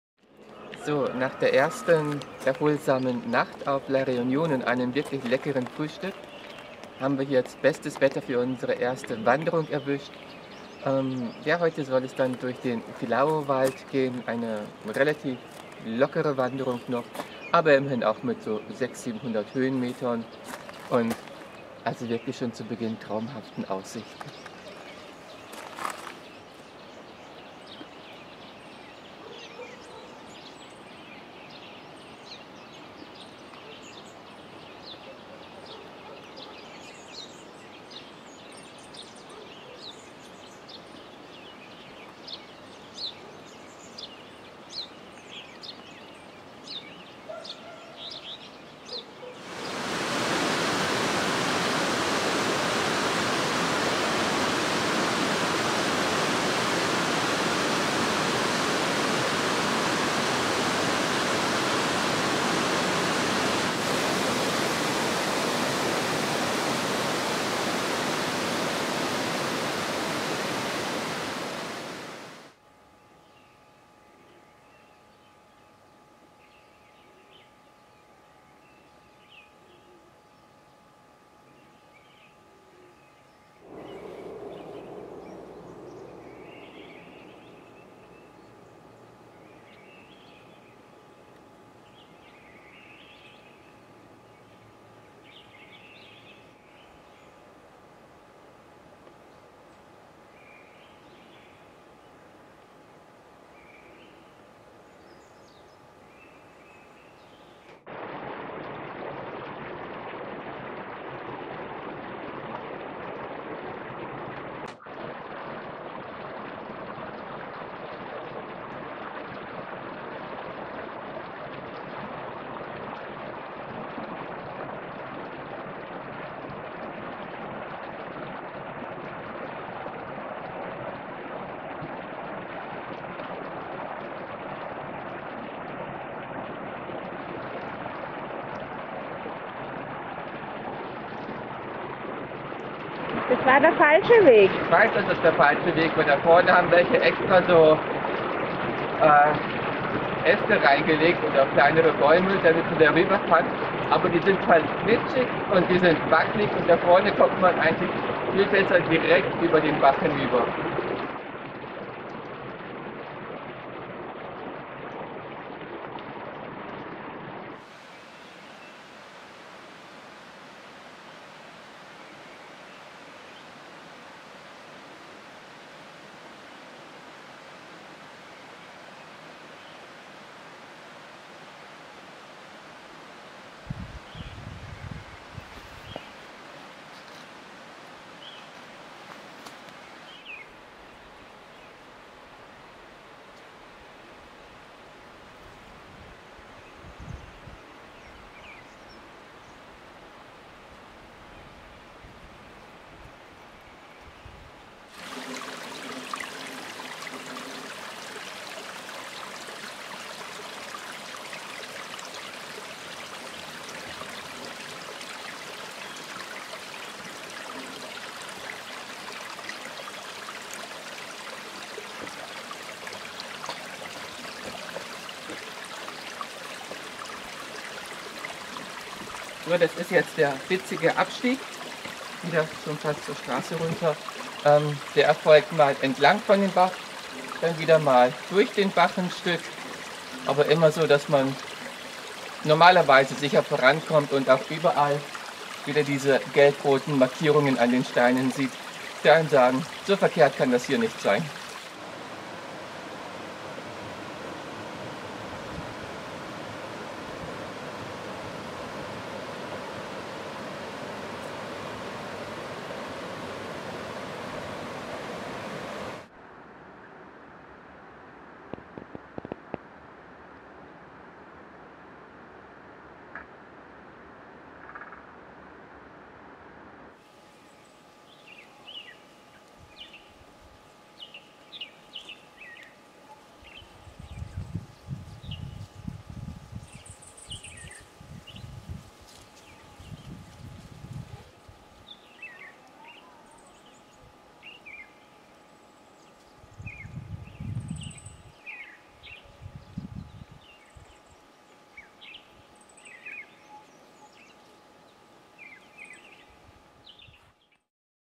Wanderung am Grand Sable im Cirque de Salazie auf Réunion
Eindrücke einer landschaftlich sehr schönen Rundwanderung am Grand Sable im Cirque de Salazie, La Réunion. Eine ausführliche Beschreibung und Karte mit gps-Daten findet Ihr